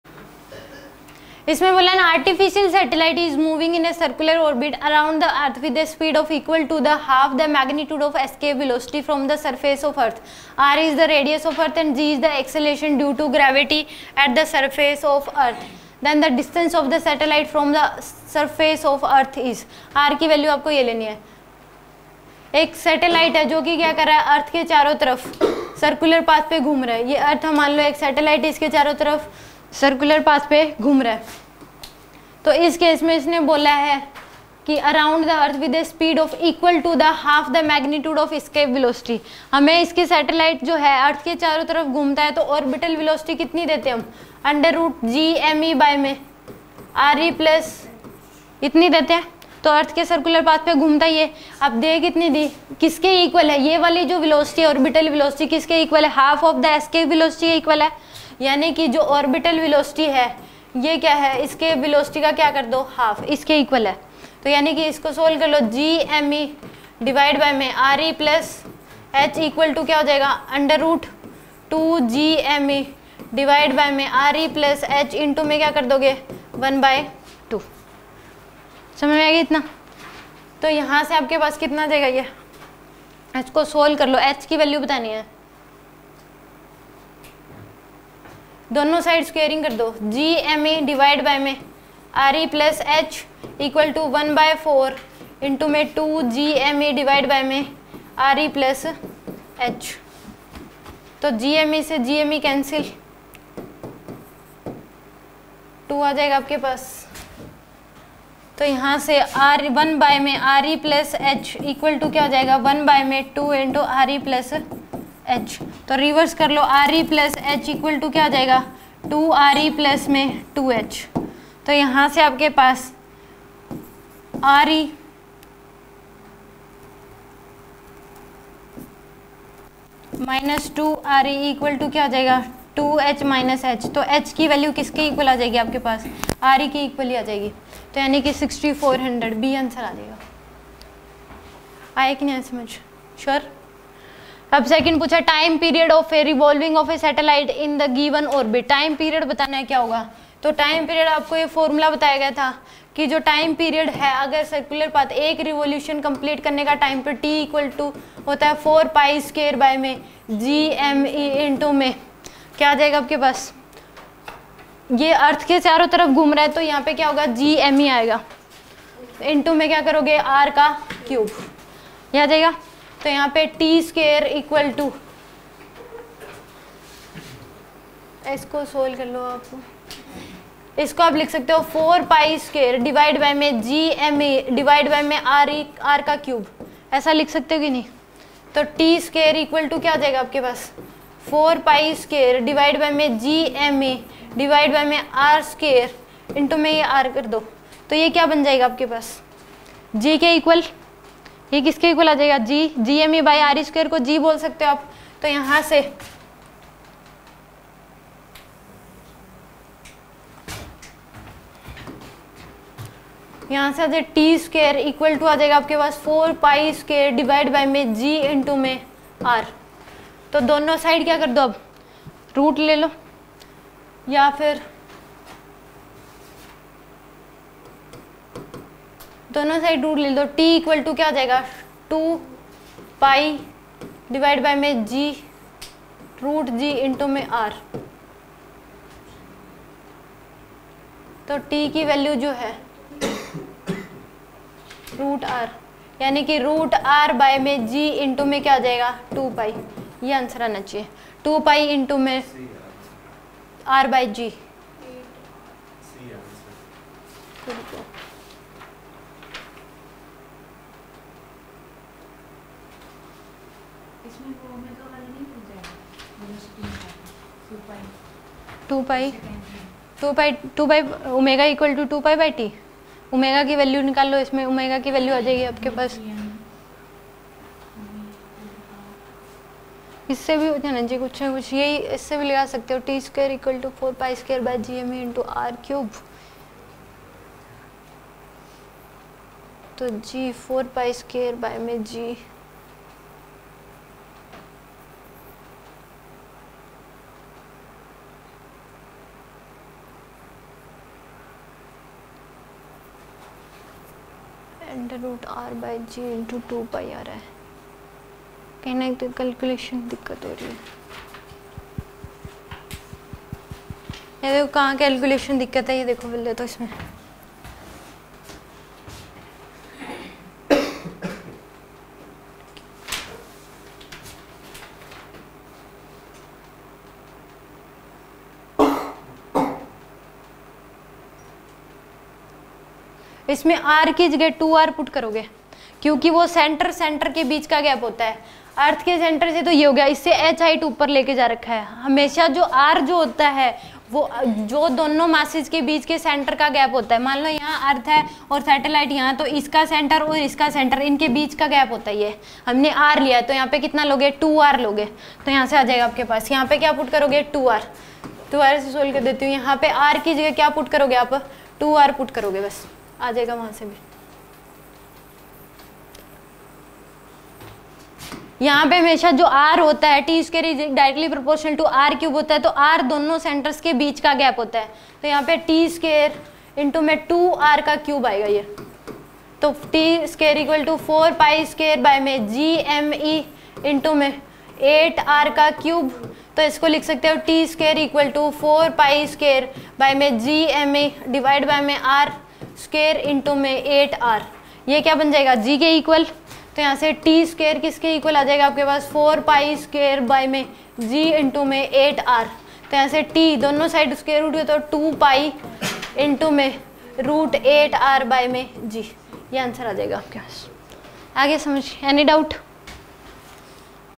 GT DPP 12 TO 14 Q34 & 35 An artificial satellite is moving in a circular orbit around the earth with
An artificial satellite is moving in a circular orbit around the earth with a speed equal to half the magnitude of escape velocity from the surface of earth. R is the radius of earth and g is acceleration due to gravity at the surface of earth.
(R=6400 km)
(JEE-Advance)
Then the distance of satellite from the surface of earth is
(a) 3200 km
(b) 6400 km
(d) 4800 km
The time period of revolution of satellite in the given orbit is
(a) 2π. 2R g
(b) 2π 4R g